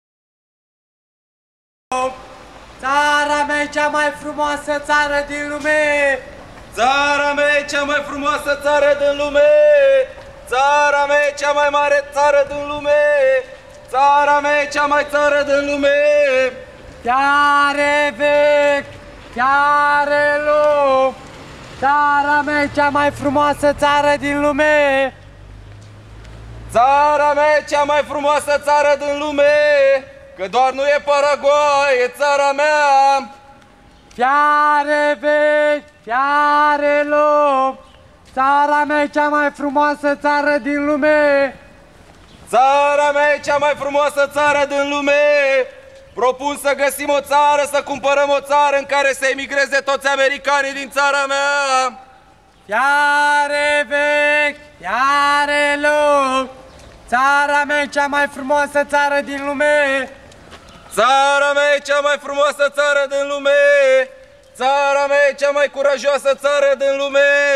AII
A(II) Rh +, de Nicoleta Esinencu, în cadrul programului Spațiul Public București (2007), inițiat de Goethe Institut București. Curatori: Marius Babias și Sabine Hentzsch.